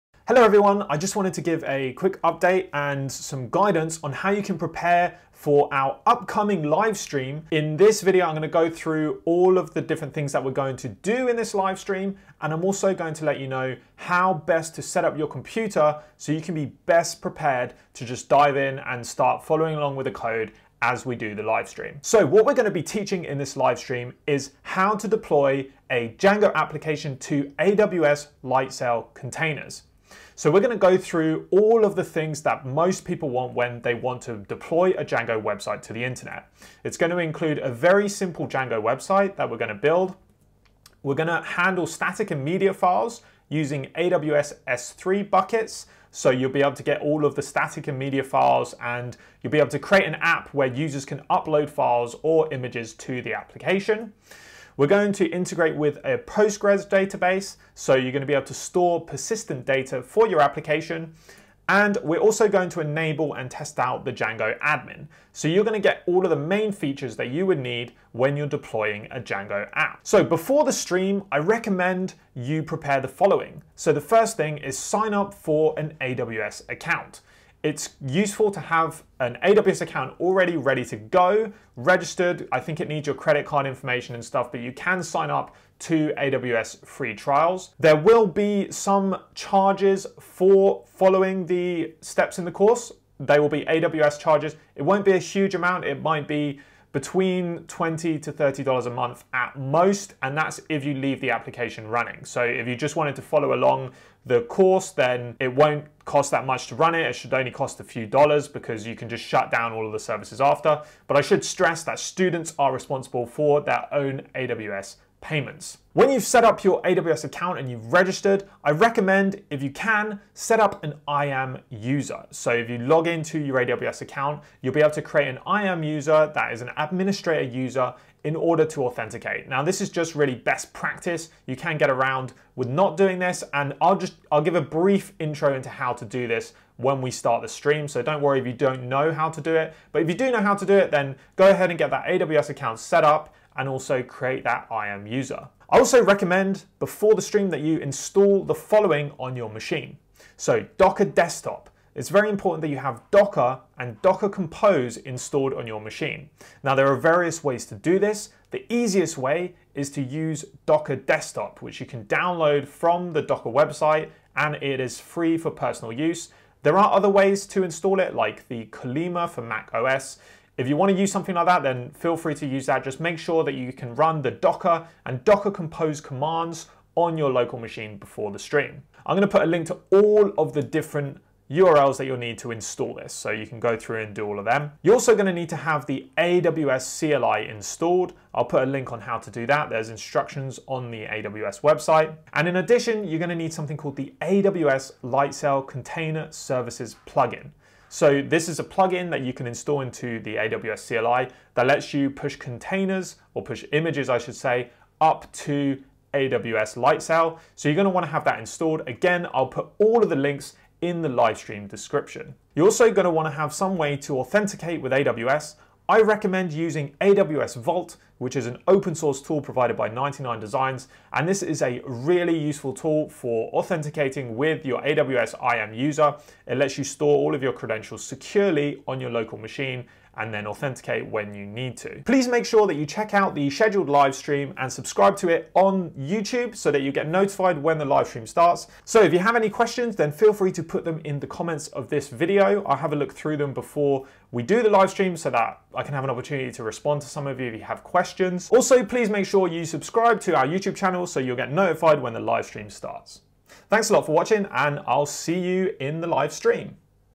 AWS Lightsail Live Stream Preparation
This video will help you prepare for our live stream tutorial on Saturday Jan 14th at 3pm at GMT. Please note, this live stream was scheduled for Dec 30th but has been re-scheduled due to covid.

To best prepare for the stream:

1. Create an AWS account
3. Install the AWS CLI
4. AWS Lightsail container services plugin